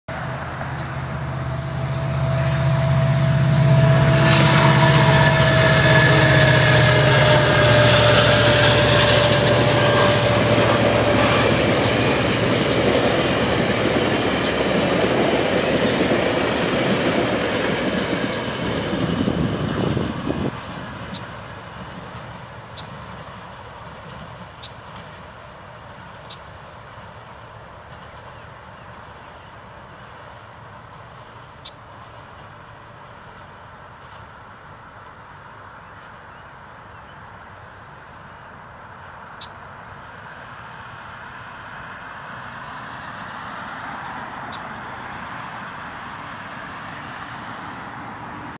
GO 641 Oakville yard May 29 2011
Just another eastbound go train with MP40 641 heading to Clarkson on this cloudy afternoon May 29 2011